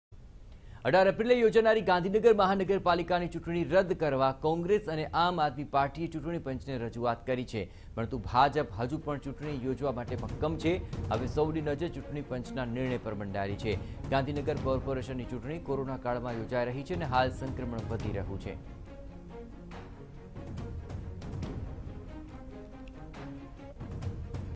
AAP, Congress ask ECI to cancel Gandhinagar polls due to rising Covid-19 cases | TV9News
AAP, Congress ask ECI to cancel Gandhinagar polls due to rising Covid-19 cases | TV9News | D55